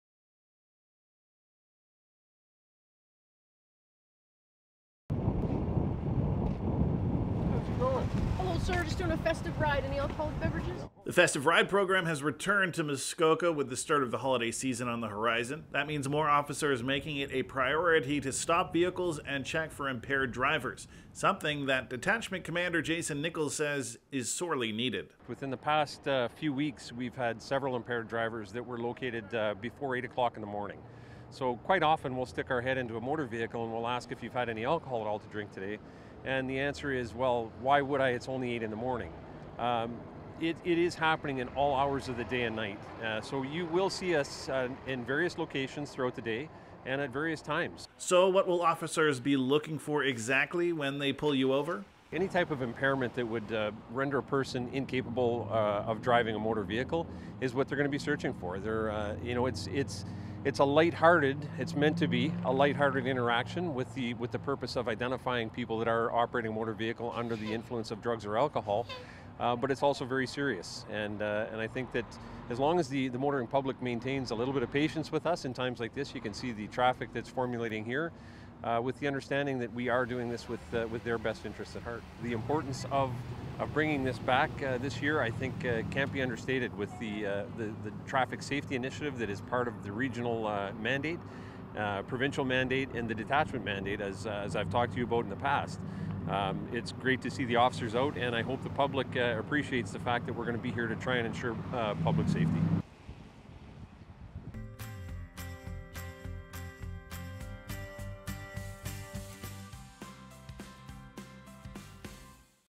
Festive RIDE campaign kicks off in Muskoka
The Festive RIDE campaign has officially kicked off in Muskoka. The OPP will be out in force across the region until January on the lookout for impaired drivers.